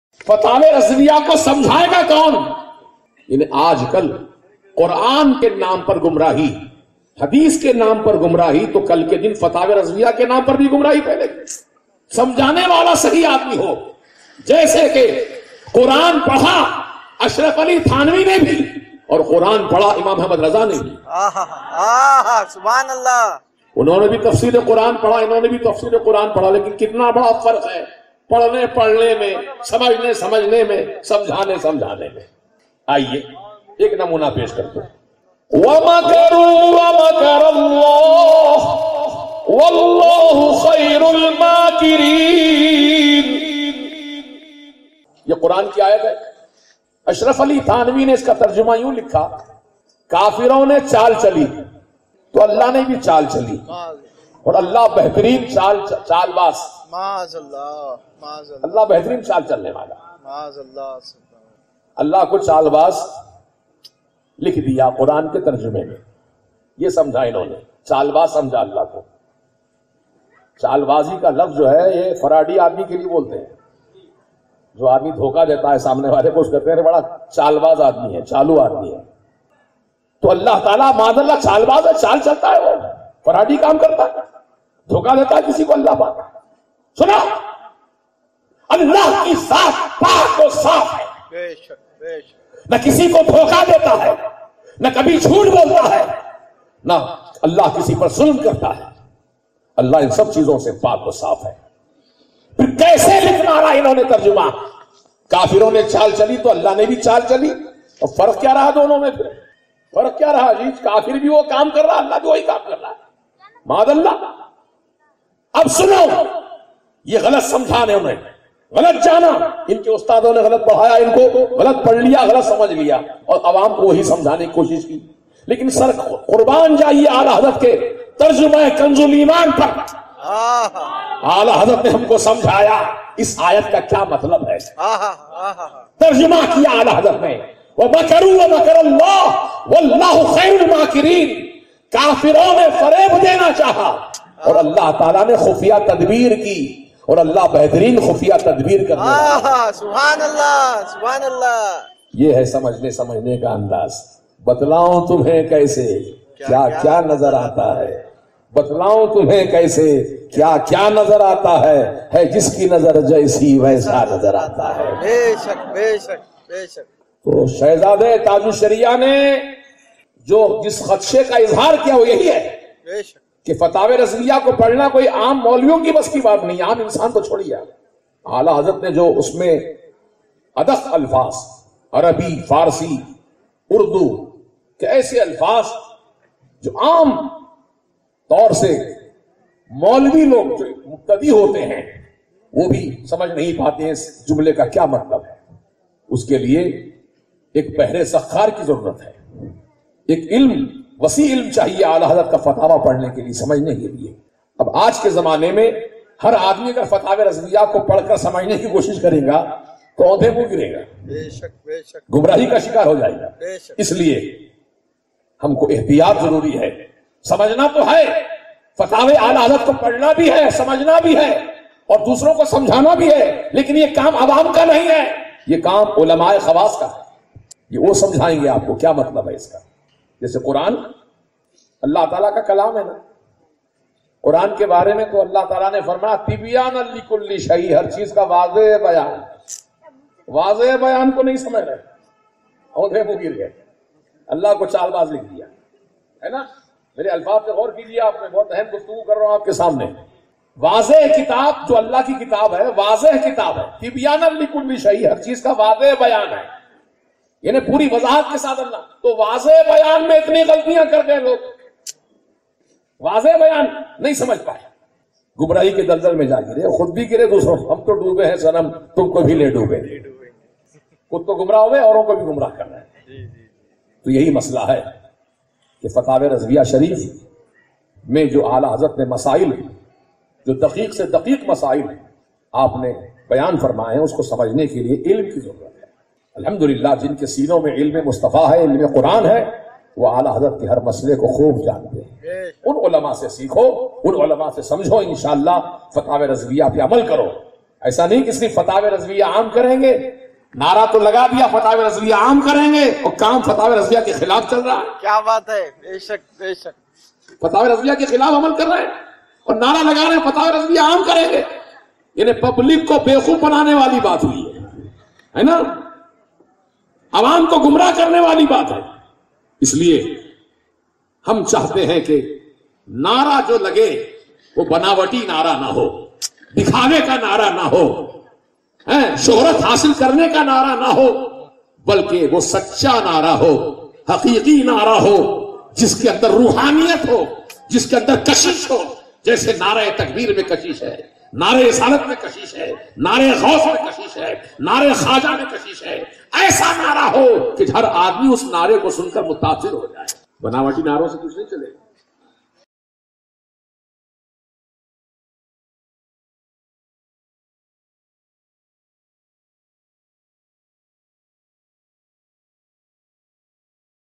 Shahzada E Huzur Tajush Sharia K Bayan Ki Himayat By Shahbaz E Dacan Allama Mujeeb Ali Razvi.
Jamat E Ahle Sunnat K Akabir Ulama Ne Shahzada E Huzur Tujush Sharia Hazrat Allama Mufti Asjad Raza Saheb Qibla K Bayan Ki Taeed O Himayat Karte Huve Mukhalifeen Ko Muh Tod Jawab Diya.